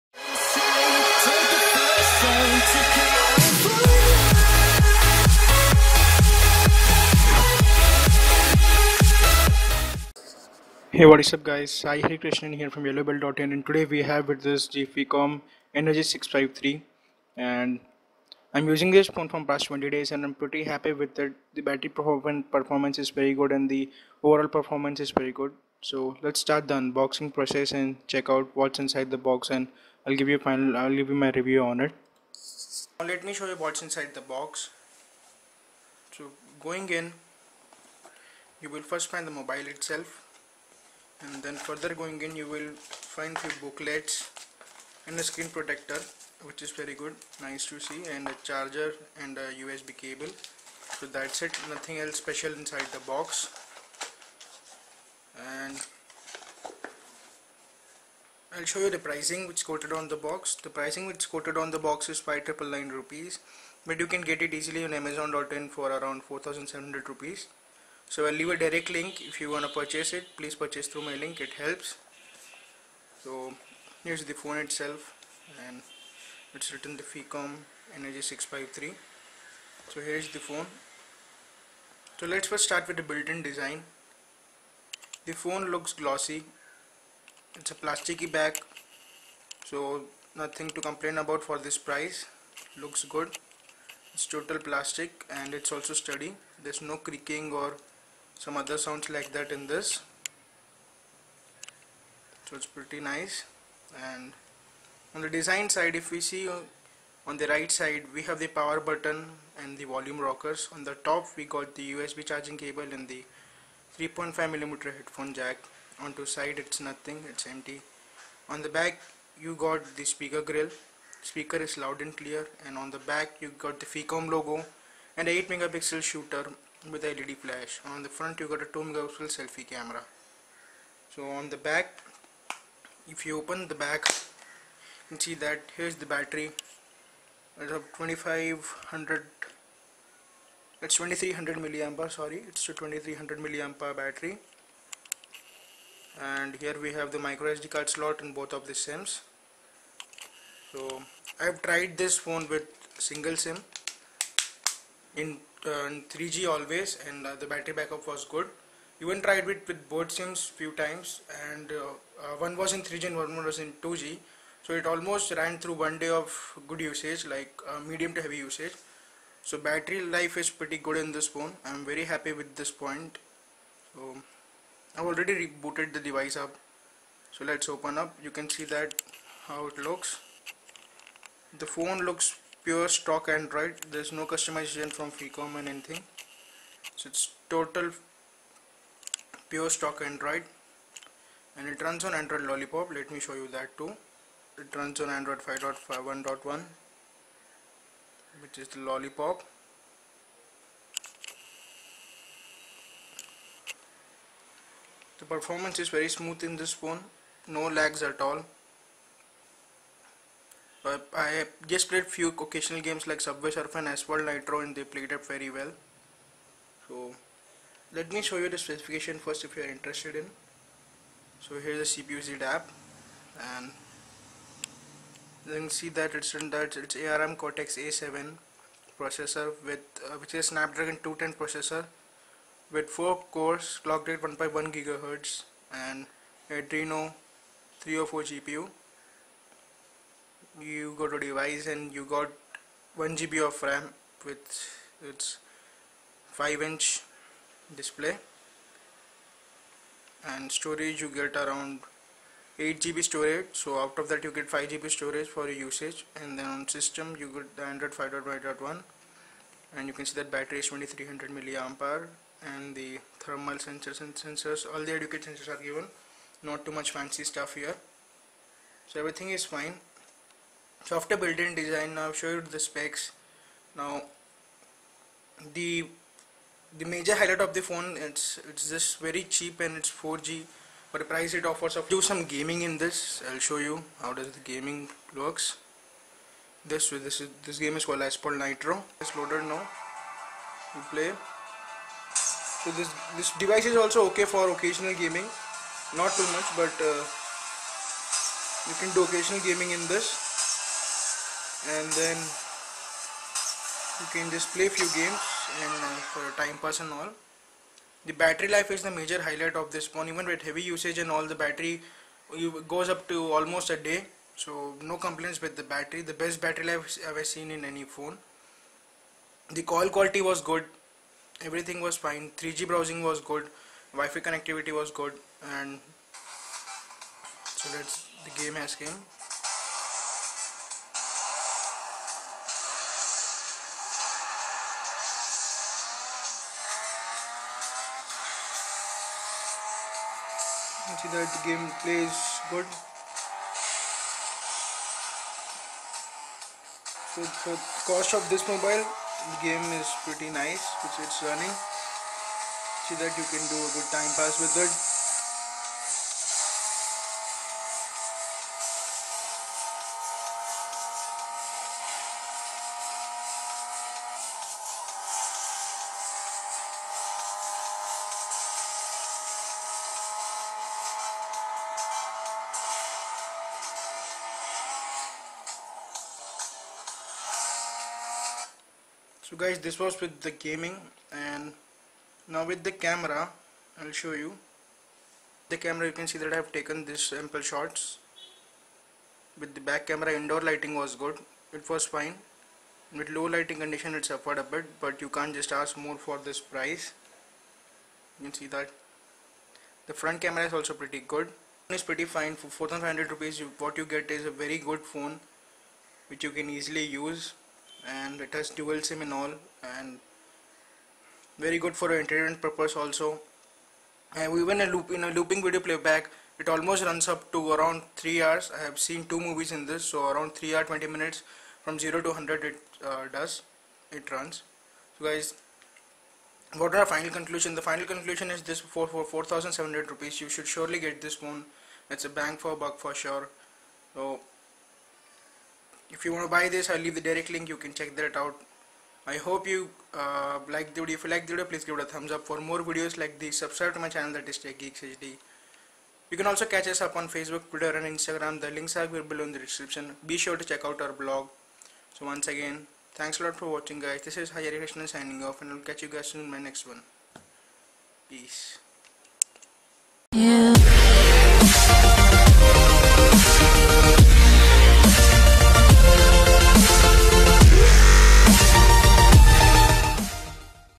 Phicomm Energy 653 - Review !! Cheapest 4G phone - Extreme Battery Life
Phicomm Energy 653 is the cheapest 4G phone you can get today
with good battery backup and decent performance .

Few specifications are

8MP primary camera with auto focus and 2MP front facing camera
It  has got a 5-inch HD IPS capacitive touchscreen with 1280 x 720 pixels resolution and it runs on Android v5.1 Lollipop operating system with 1.1GHz Qualcomm MSM8909 quad core processor, 1GB RAM, 8GB internal memory expandable up to 64GB and dual SIM and 2300mAH lithium-ion battery

This phones is surely Bang for buck , You can buy this as your secondary backup device , or can gift it to someone ,
you can Buy this amazon india ,below is the link

Install Flipkart App

Hope you guys enjoyed the video please

Peace.